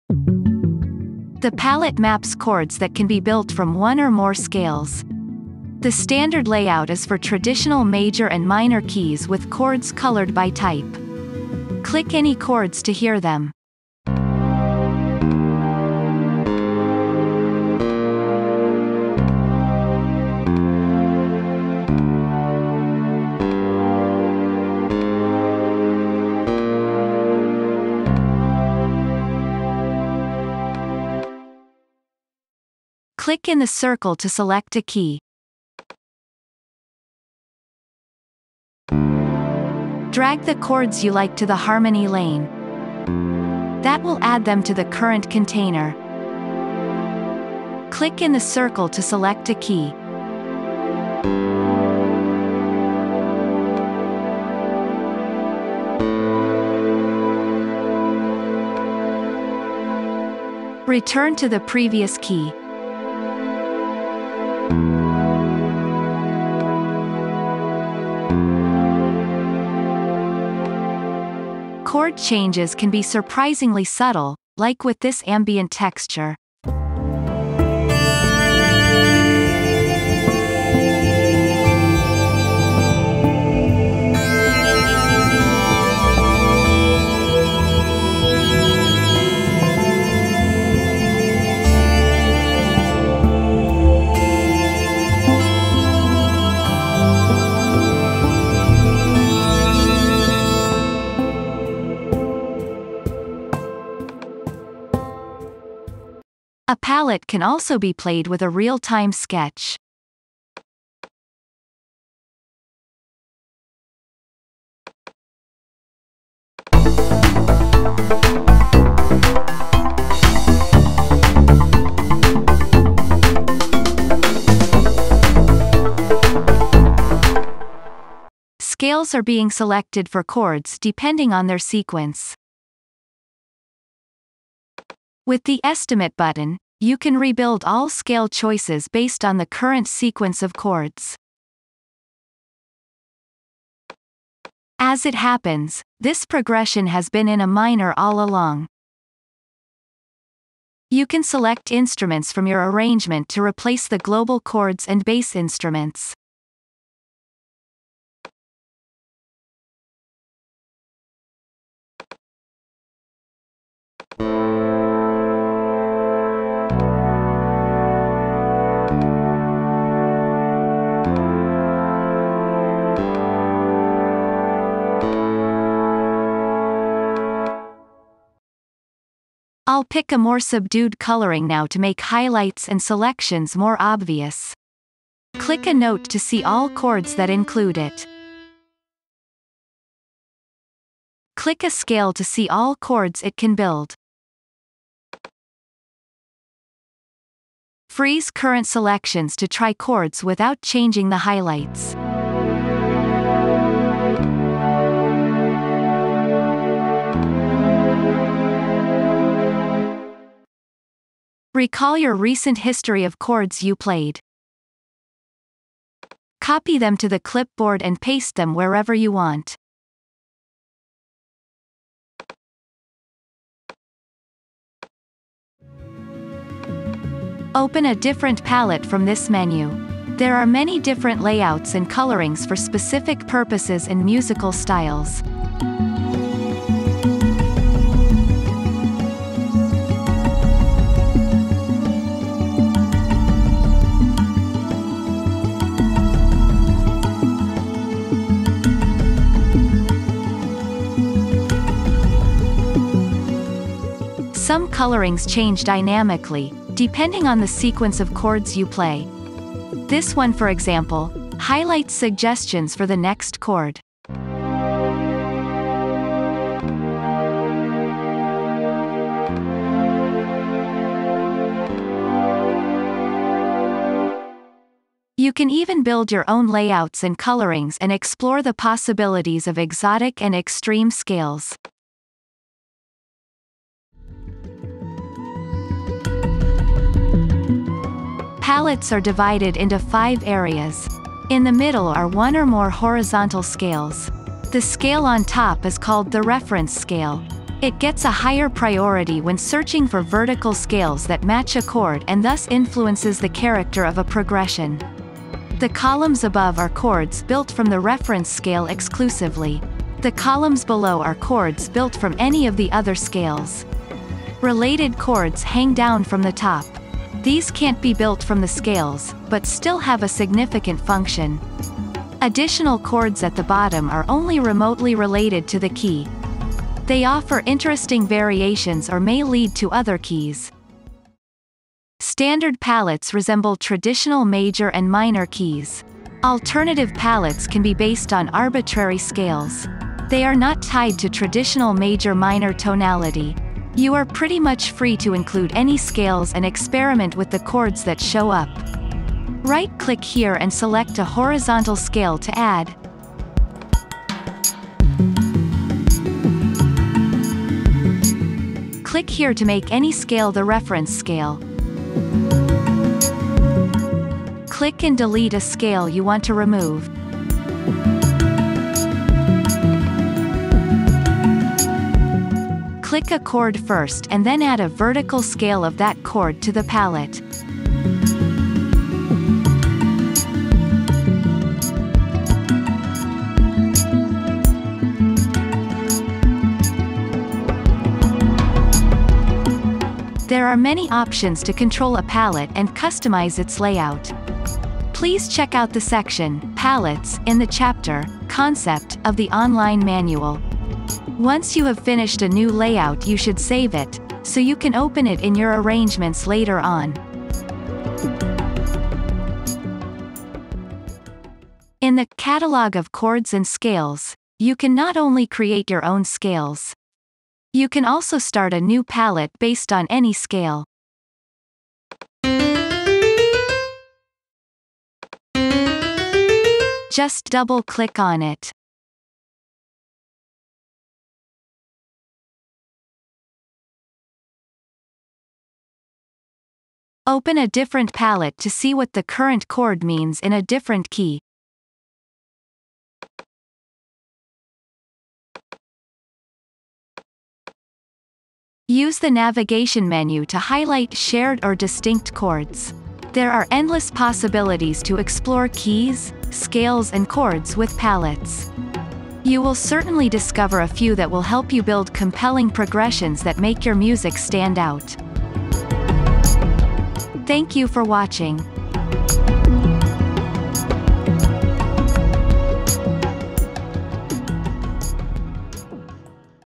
Harmony (2): Palettes
Learn how to use the Palette to navigate keys, build chord progressions and explore the space of harmony with your own Palette layouts. Browse and discuss all tutorials in context on our community